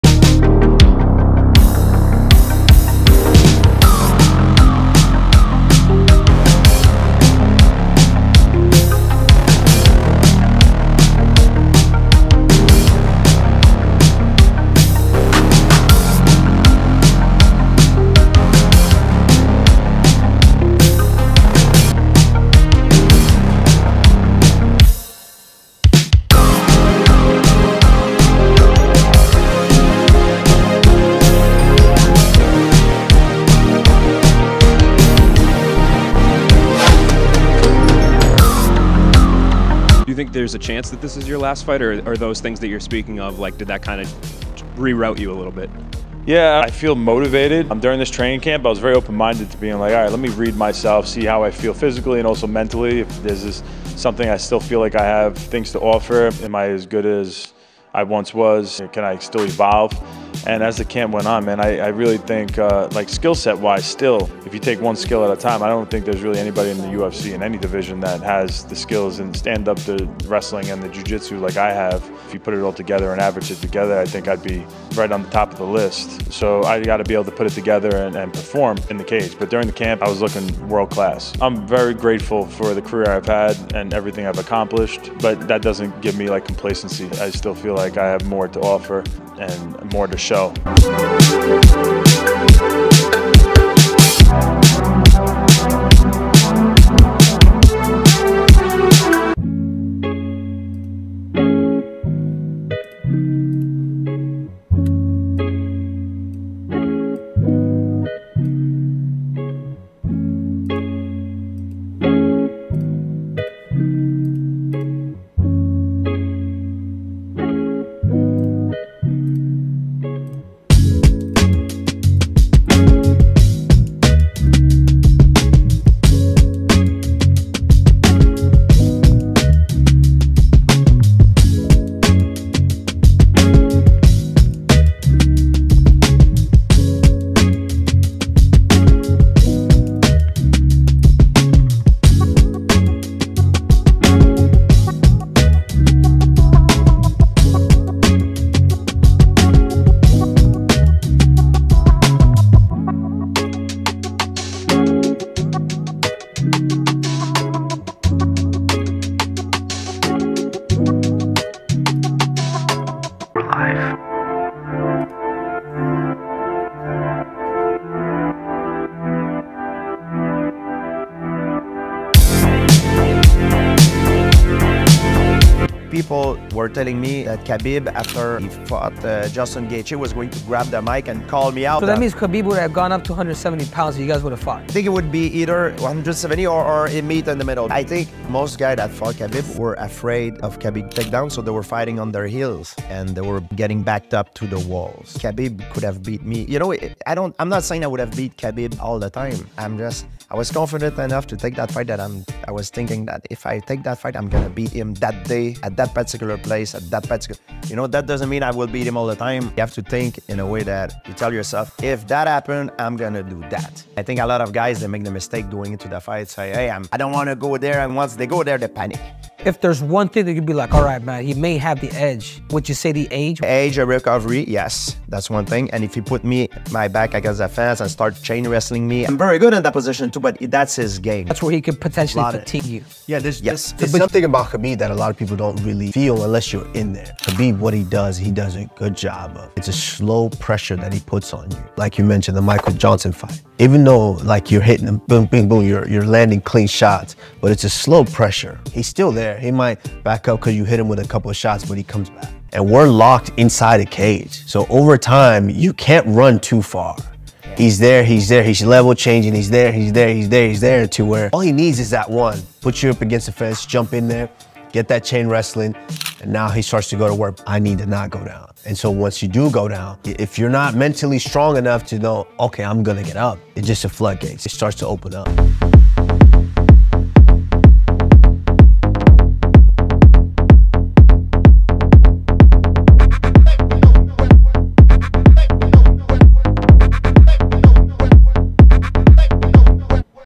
GSP & Kamaru Usman Speak On Khabib Fight, Chris Weidman Speaks On His Future, Winner Fights Dricus!
0:00 - Intro
0:05 - UFC In Saudi Arabia Main Event Winner Takes On Dricus Du Plessis Next For Title
0:20 - Sean Strickland Turns Over A New Leaf
0:35 - Chris Weidman Speaks On His Future In This Sport
1:31 - Ryan Garcia Continues Going Off On Twitter
1:56 - Chito Chimes In On Twitter
2:11 - Dustin Poirier Calls For Title Shot
2:21 - Muhammad Mokaev Wants To Return To Action ASAP
2:36 - Charles Oliveira Is Having A Son
2:46 - GSP & Kamaru Usman Speak On The GSP vs. Khabib Matchup
4:35 - Guess Who This Former Champion Fighter Is
4:45 - Like & Subscribe